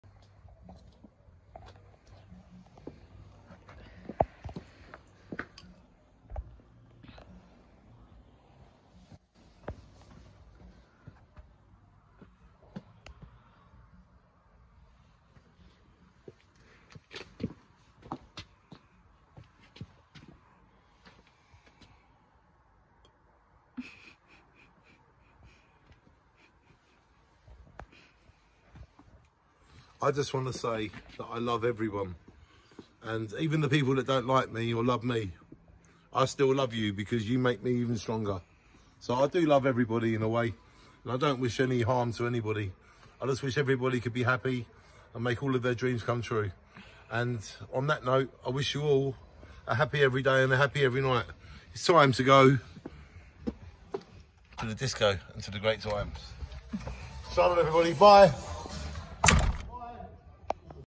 This film is my message to all people that like & don’t like me because I’m so grateful to you all
This film is my message to all people that like & don’t like me because I’m so grateful to you all for the way you are,shine on. DannySloggettVision